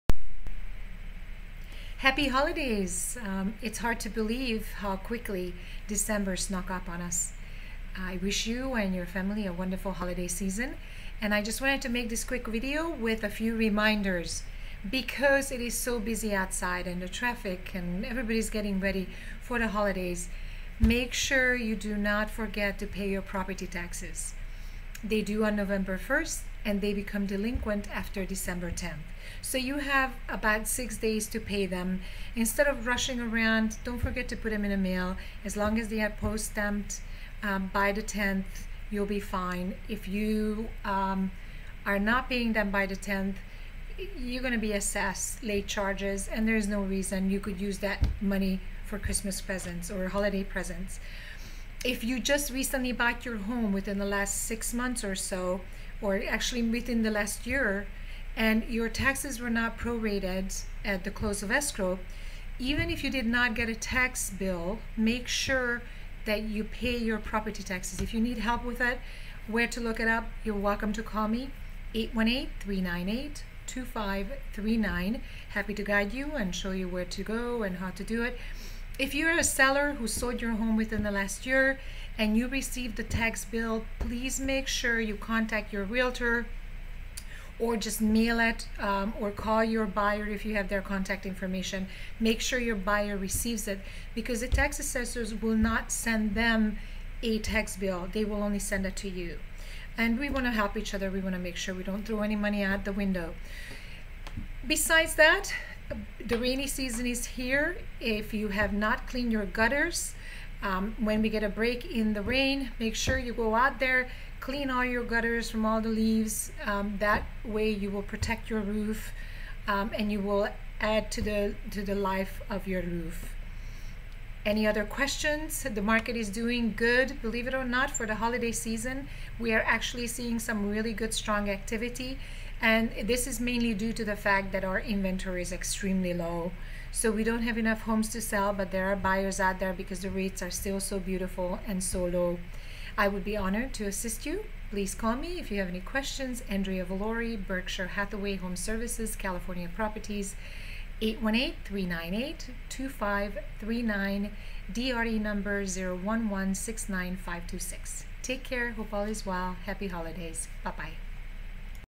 Important Property Tax Info...
Info about Property Tax. Everyone should know and listen to before December 10th. Call me with any questions
ANDREA VOLORE
Berkshire Hathaway Home Services CA Properties DRE #01169526